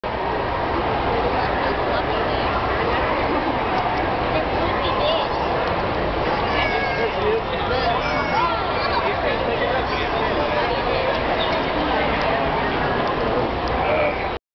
Alaskan Malamutes at the 34th Annual Houston Reliant Park World Series of Dog Shows
Giving the 'mute a tummy rub.